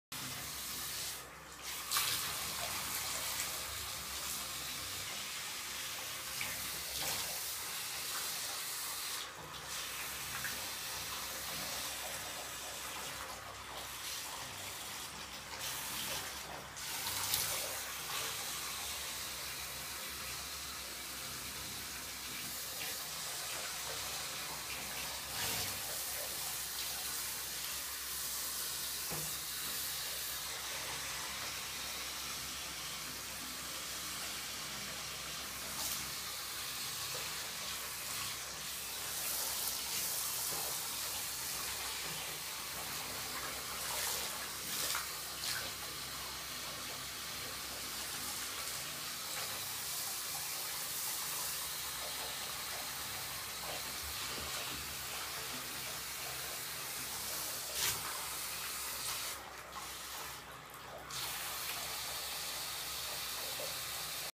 Maine coon VS shower
Мейн кун (2 года, 5 месяцев) в душе.